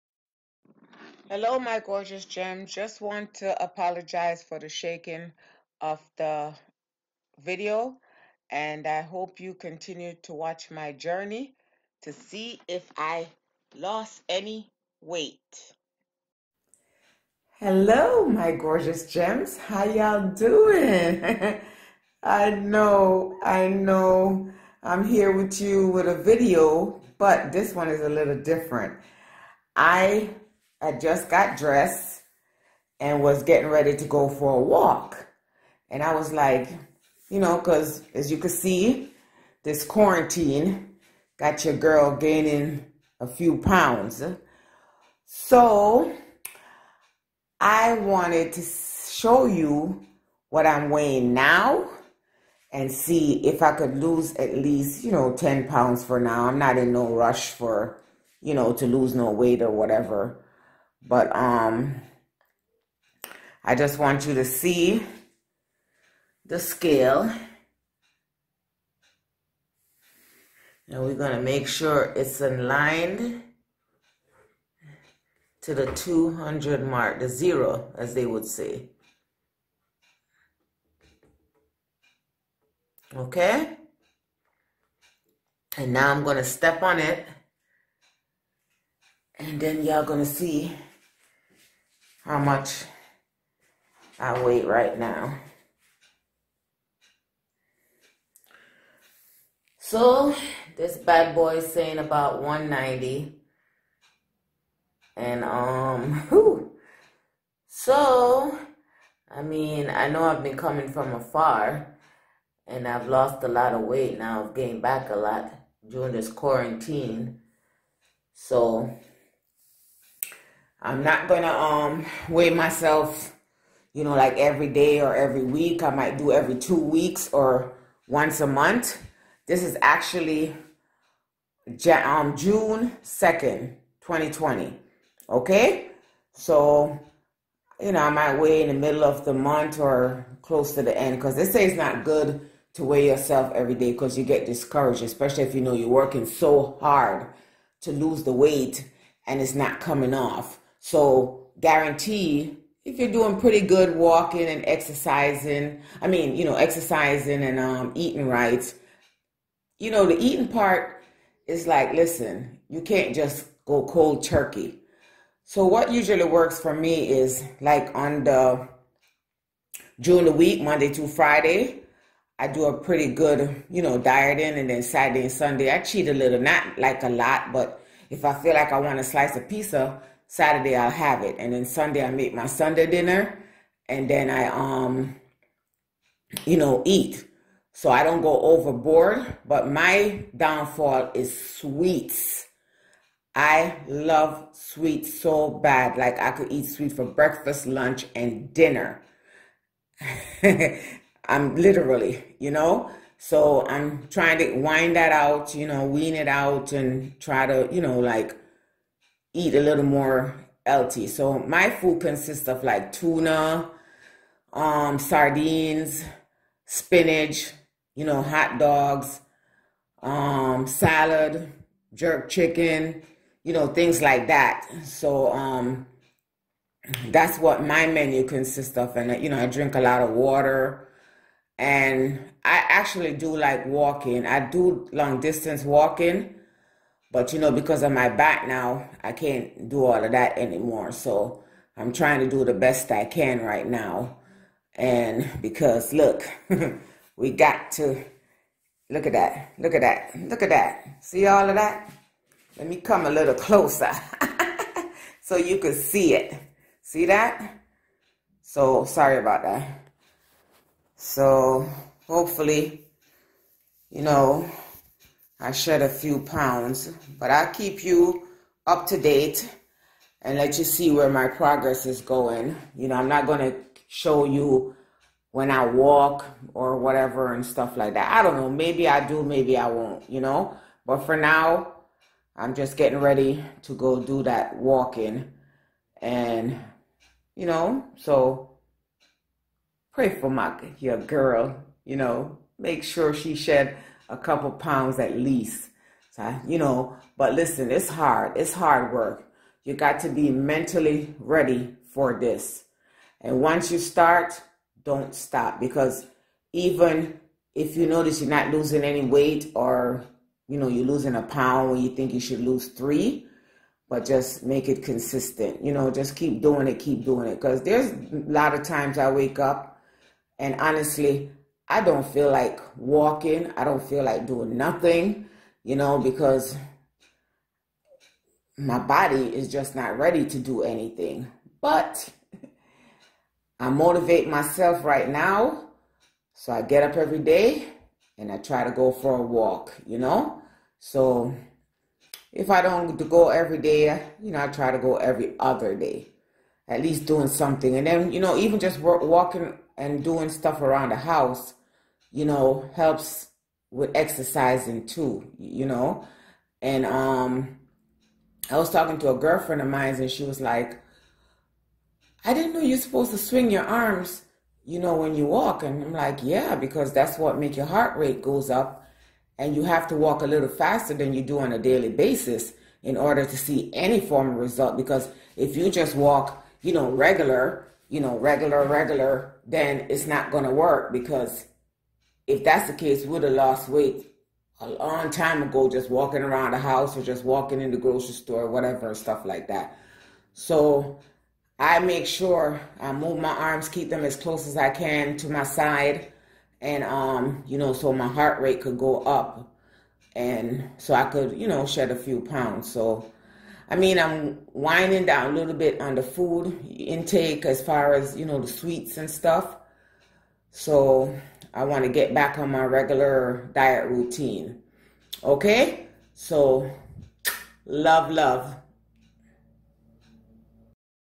COME ALONG WEIGHT LOSS JOURNEY
Hello my gorgeous gems glad I got this scale from the goodwill now I can keep up with my weight loss on this  quarantine